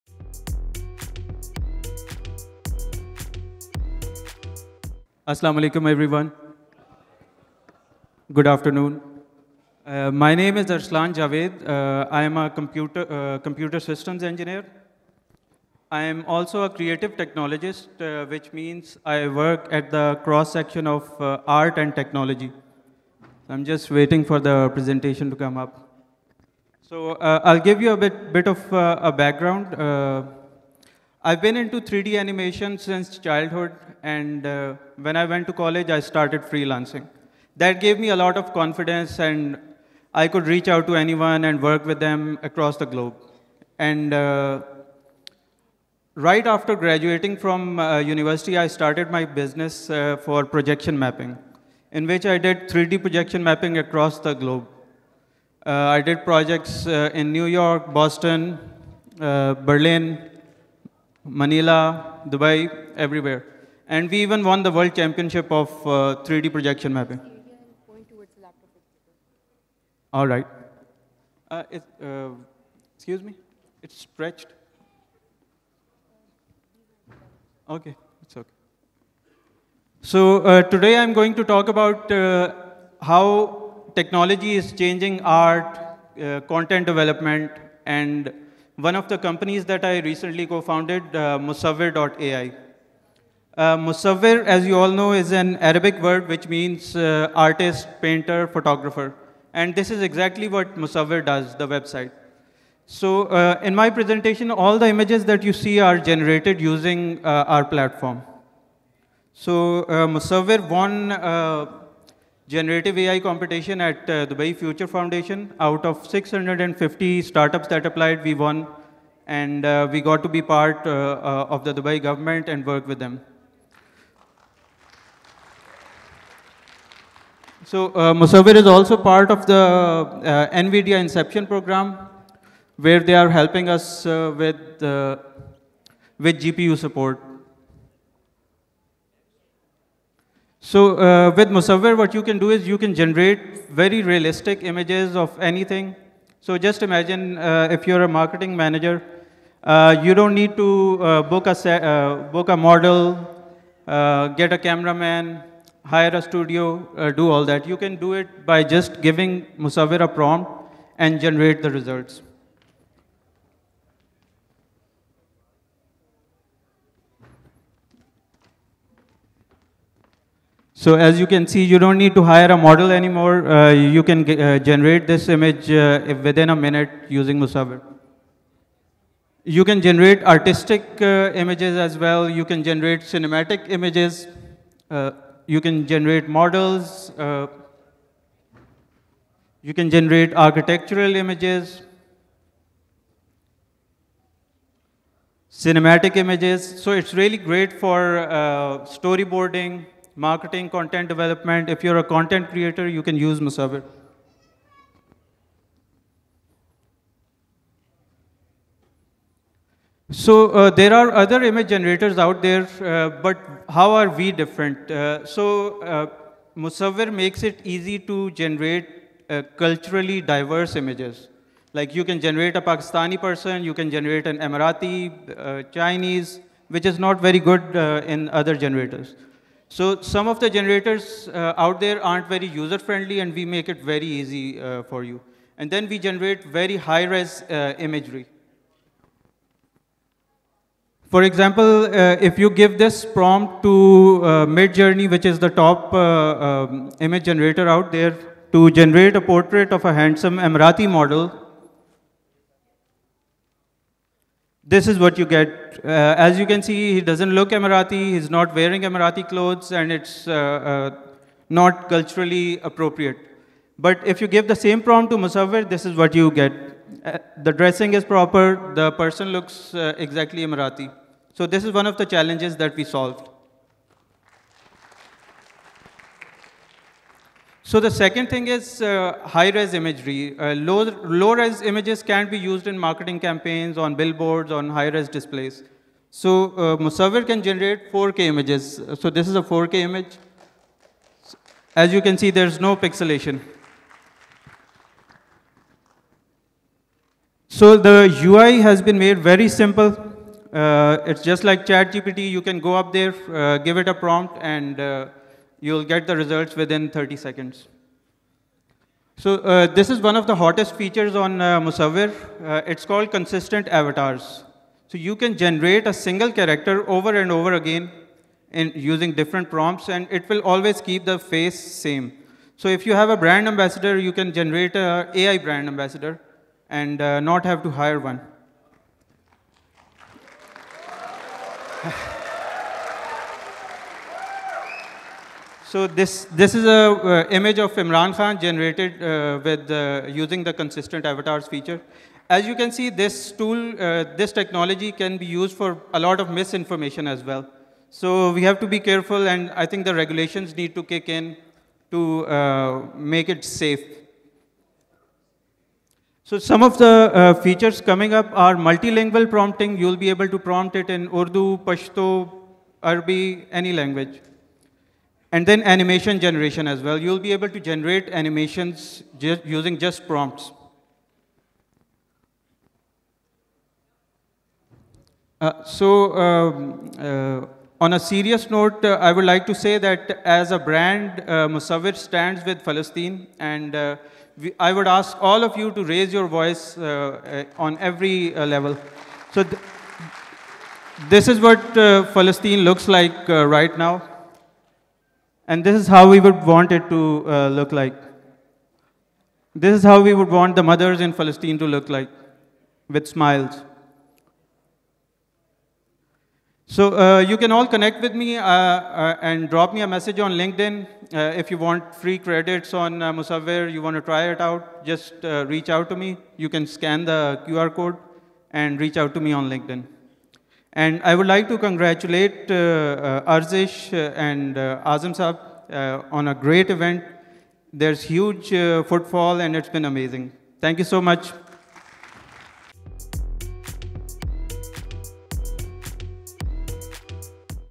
Arslan Javed keynote at Future Fest 2024 | Ejad Labs
Arslan Javed, Co-Founder, Musavir.AI  | Graana During his keynote at Future Fest 2024. He talks about:

From Freelancer to Founder: 🌟 Arslan recounts his transition from freelancing in 3D animation to founding a groundbreaking AI-driven content creation company.

Impact of Technology on Art: 🖌️ Exploration of how MAV leverages generative AI to transform artistic and content creation processes.

MAV's Capabilities: 🖥️ Highlighting MAV's ability to produce high-resolution, culturally diverse images suitable for various applications.

Innovative Features: 👤 Introduction of the "consistent avatars" feature, enhancing brand consistency across digital campaigns.

Regulatory Concerns: ⚖️ Discussion on the potential risks of AI in generating misinformation and the urgent need for appropriate regulations.

Support for Palestine: ✊ Arslan expresses solidarity with Palestine, encouraging viewers to advocate for justice and peace.

Engagement and Offers: 🎁 Offering free credits on MAV and extending an invitation to connect on LinkedIn for further collaboration.